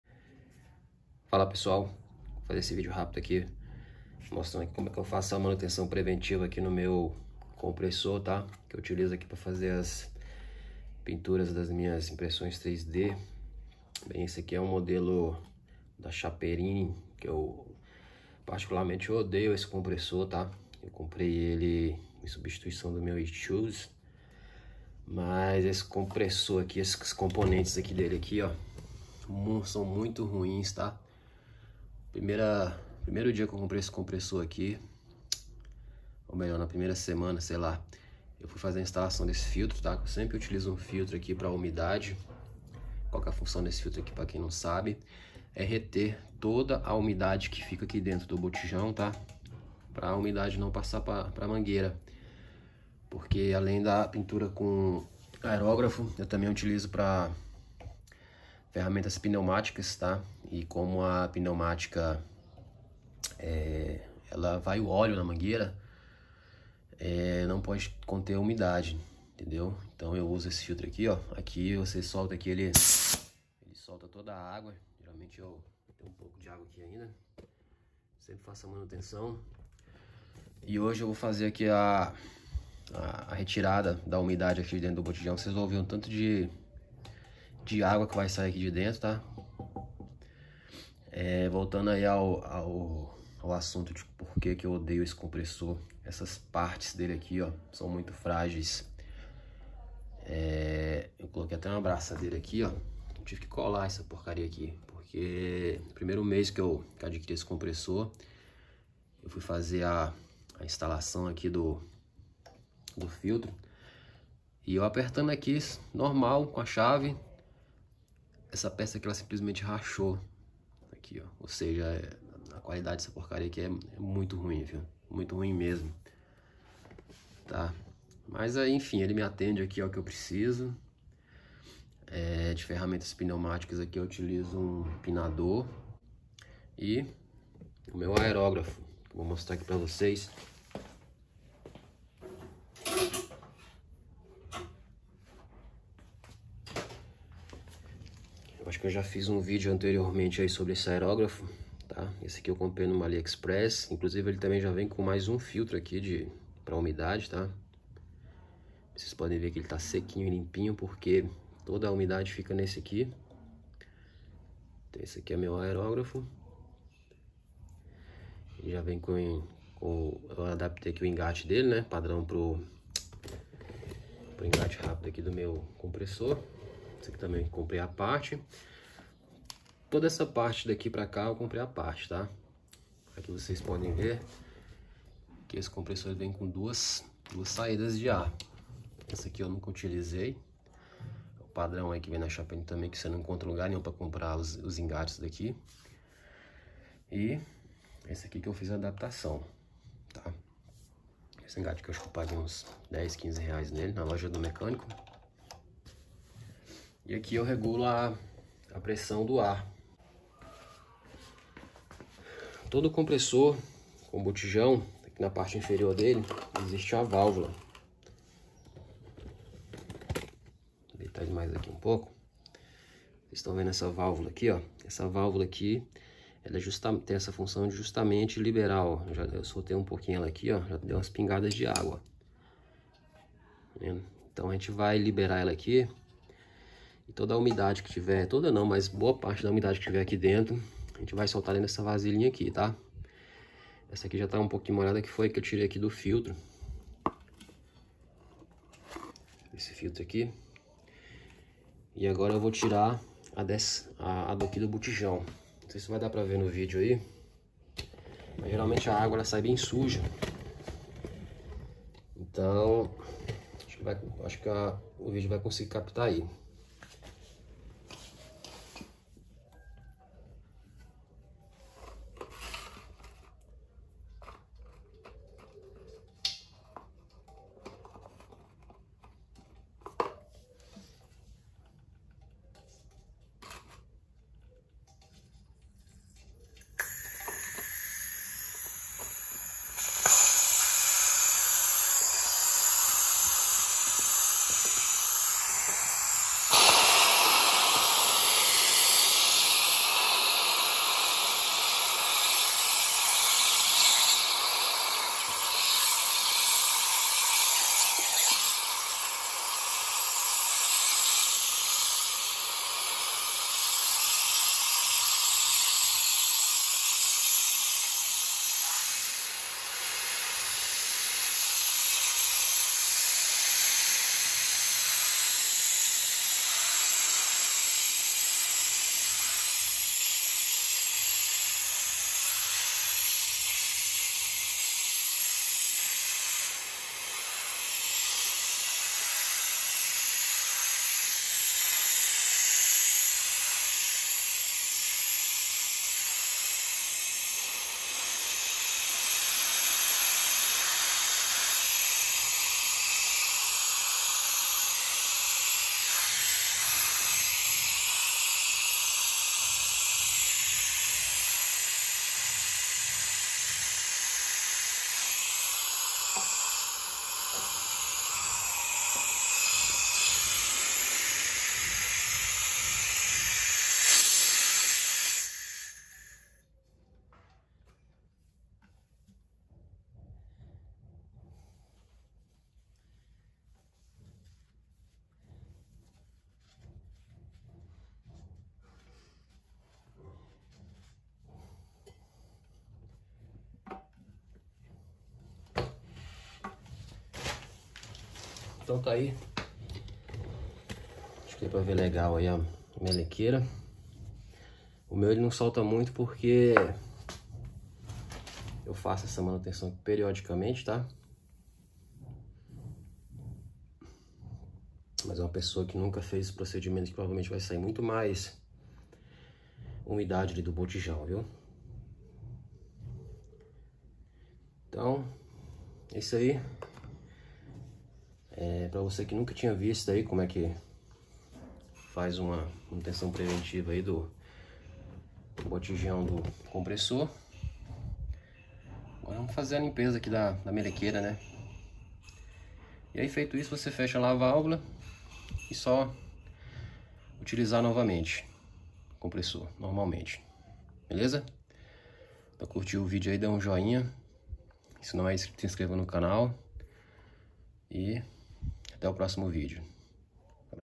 Manutenção preventiva compressor chiaperini (retirada de umidade do reservatório de ar) parte 1/2
Neste vídeo irei mostrar como aumentar a vida útil do seu compressor.Uma coisa que quase ninguém faz é a retirada de água que fica acumulada no reservatório de ar do compressor. Confiram!
Link do filtro
Link para o óleo